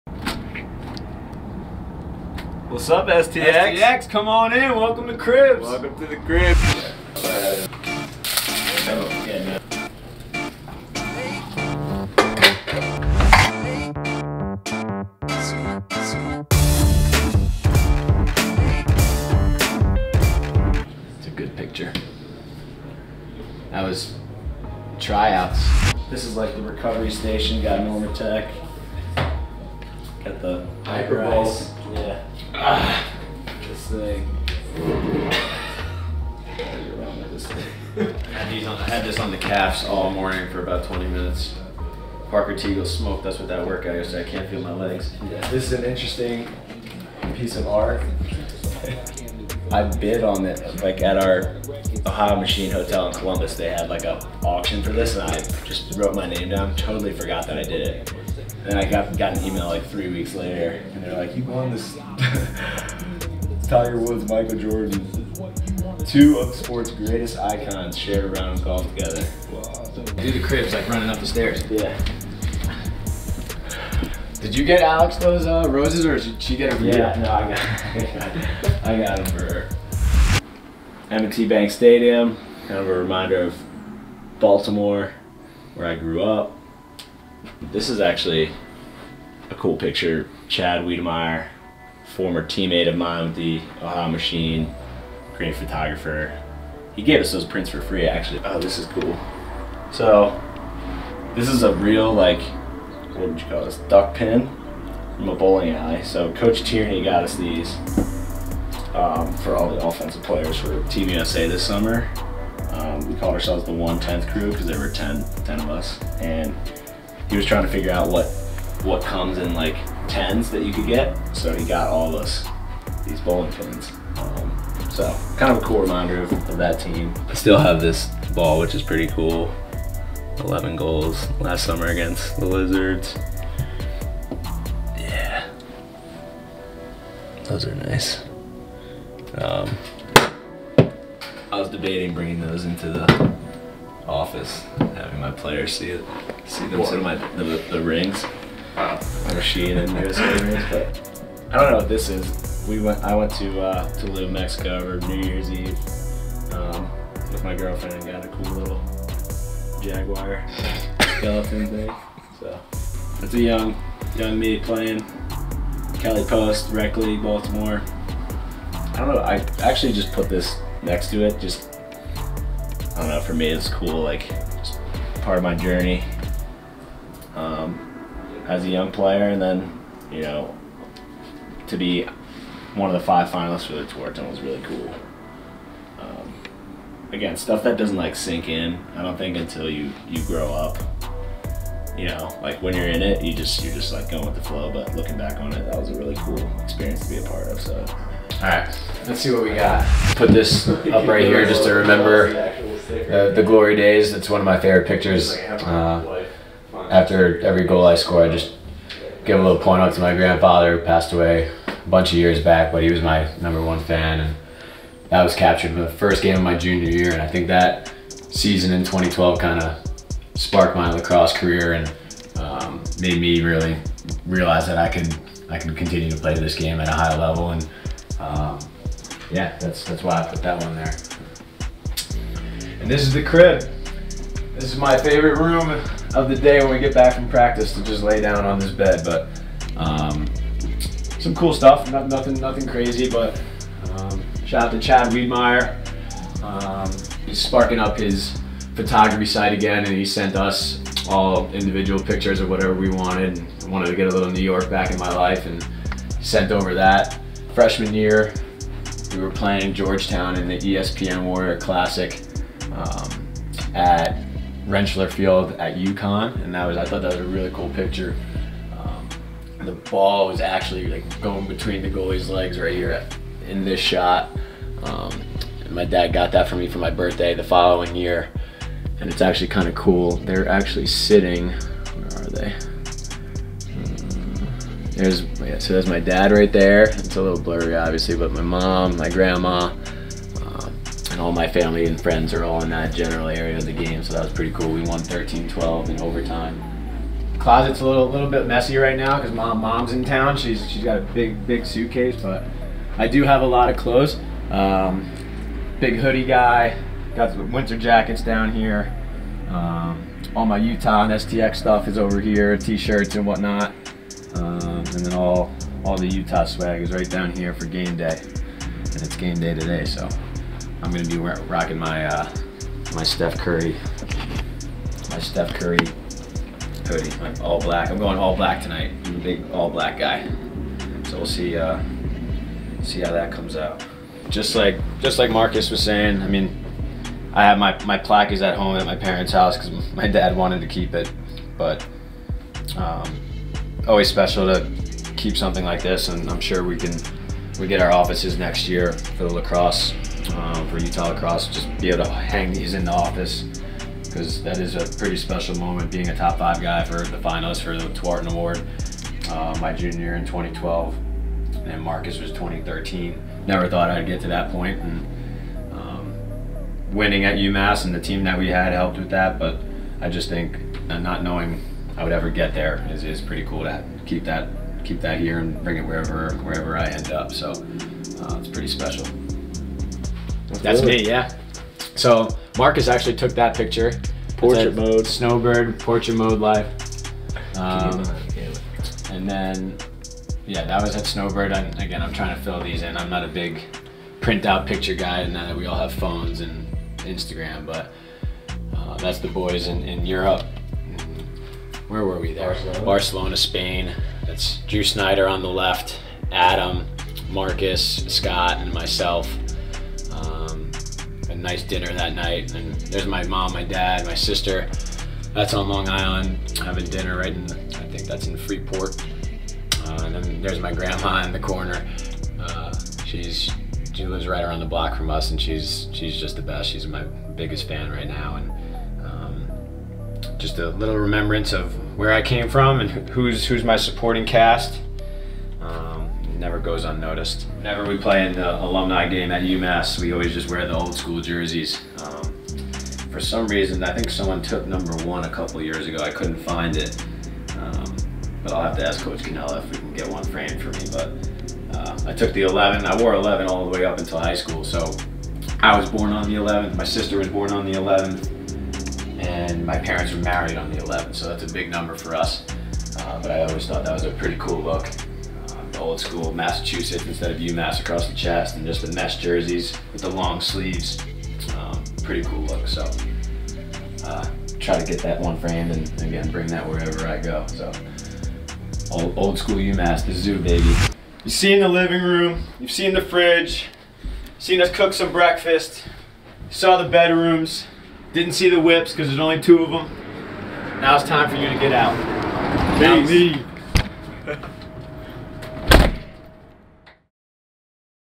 Cribs: Marcus Holman & Will Manny
PLL athletes Marcus Holman and Will Manny welcome STX into their crib for the day and show us around.